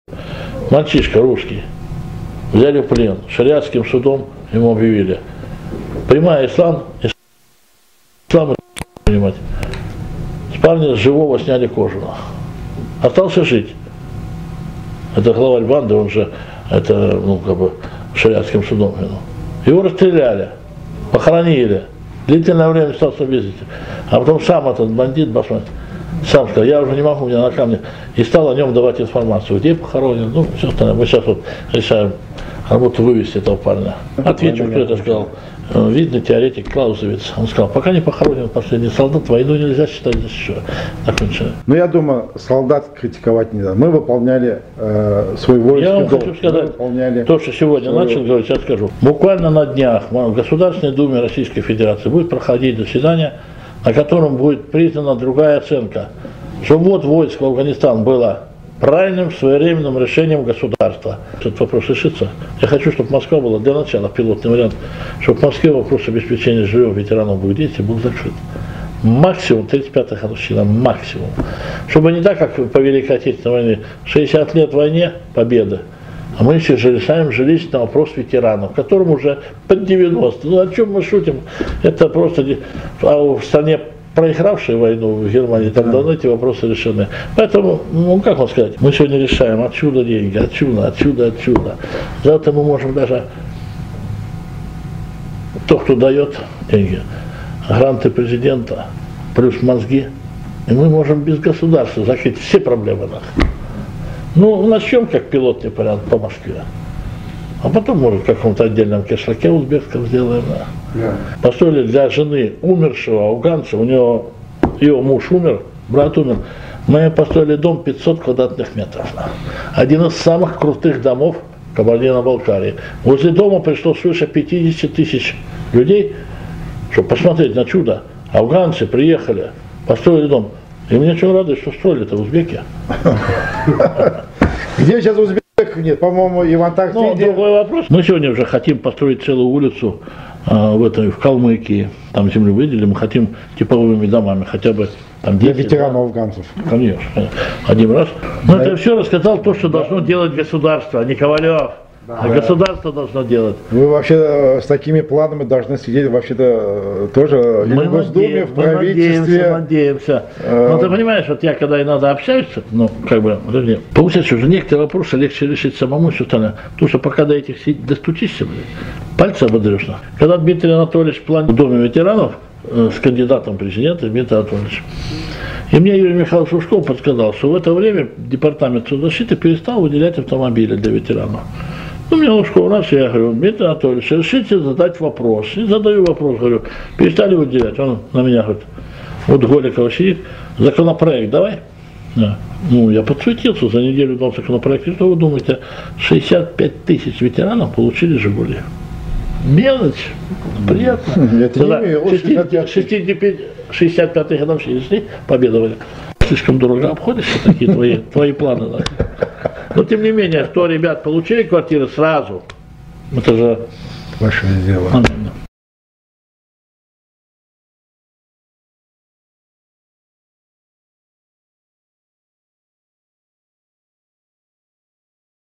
С живого солдата содрали кожу! Анонс интервью
С живого солдата содрали кожу! Анонс интервью Председателя Комитета по делам воинов-интернационалистов при Совете глав Правительств государств СНГ Ковалёва Александра Михайловича перед поездкой в Узбекистан на 1-й выездной Слёт посвящённый 30-летию окончания войны в Афганистане, данное президенту Межрегионального узбекского землячества "Ватандош" Баратову Усману Акрамовичу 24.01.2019г. г.Москва. Полная часовая версия выйдет позже.